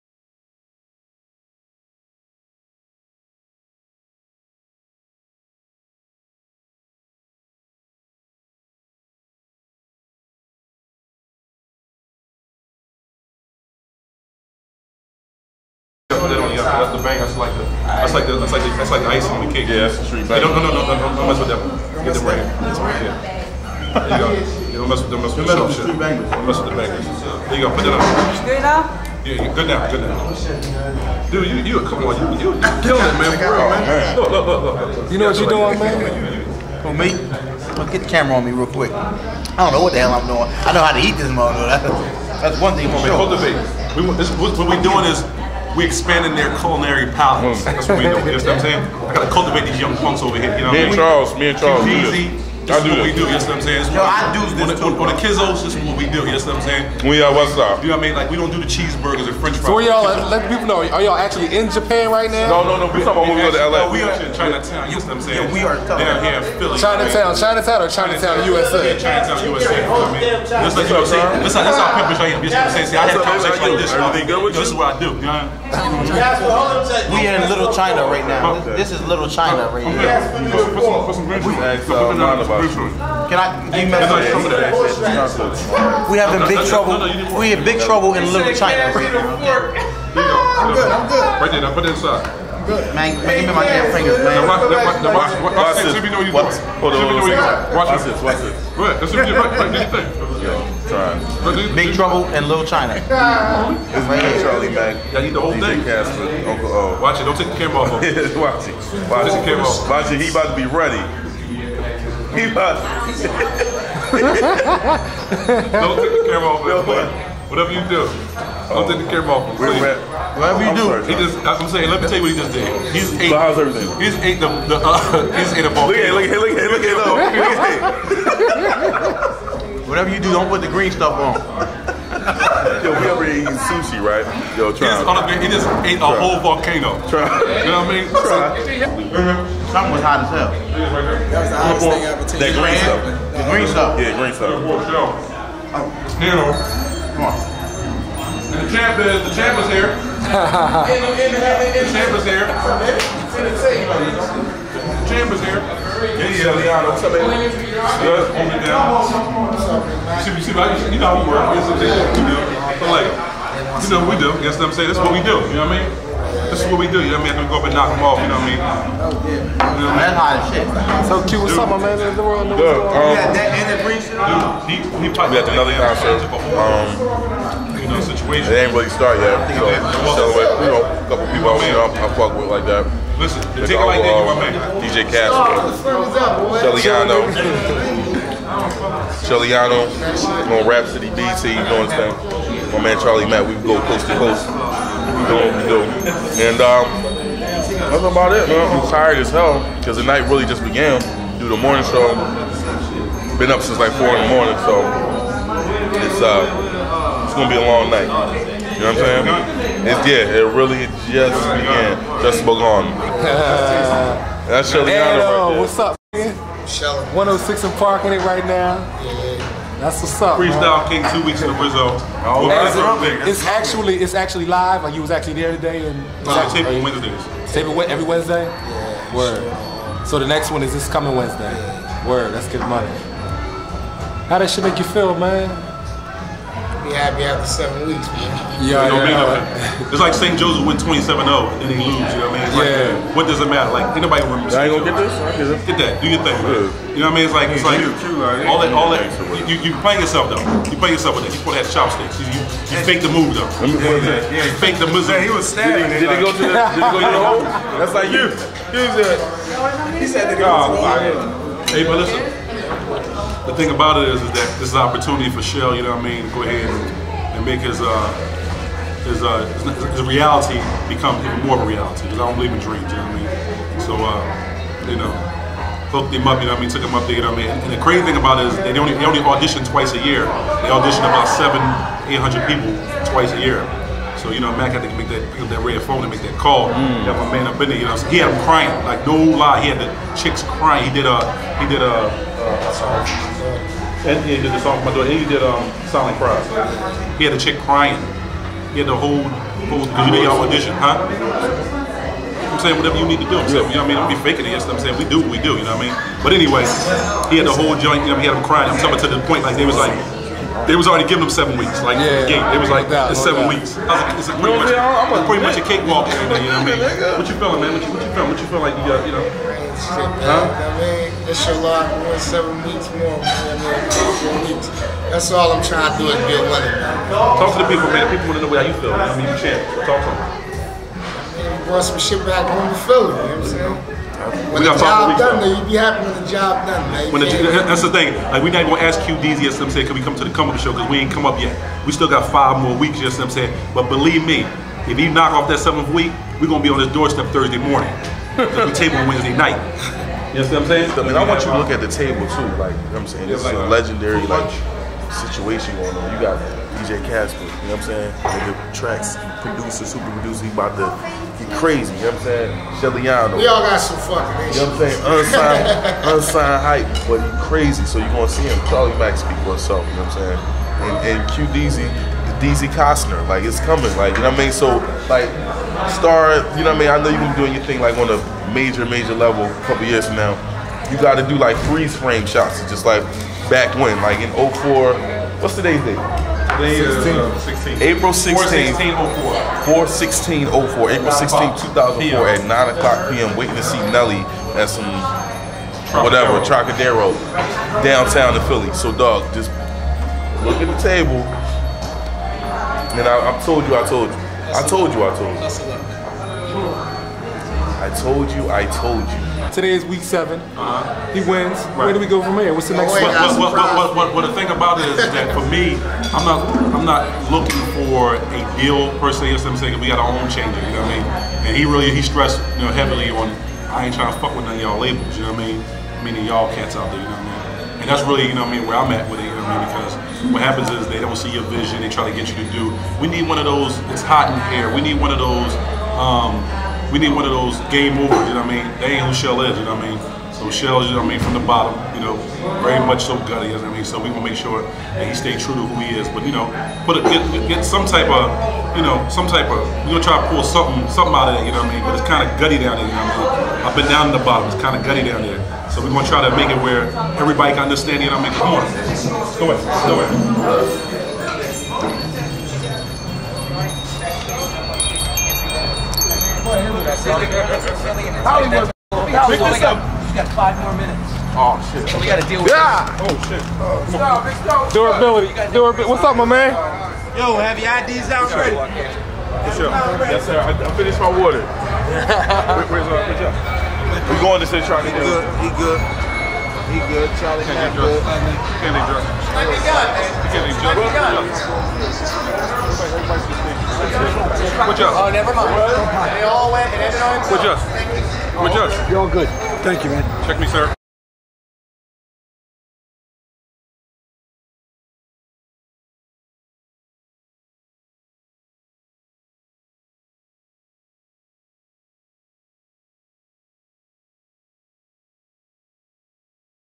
SHELLIANO, CHARLIE MACK, Q-DEEZY, DJ CASPER, UNCLE O (2003) Talking Hip-Hop
This is a rare clip I found in the vault from around 2003 during the time Shelliano had his run on 106 & Park's Freestyle Friday (BET).  On this particular night I was hanging out with Q-Deezy, Charlie Mack, DJ Casper, Uncle O, and Shelliano before a Nelly Concert.  There was no particular topic, just some brother's hanging, eating sushi, and talking hip-hop!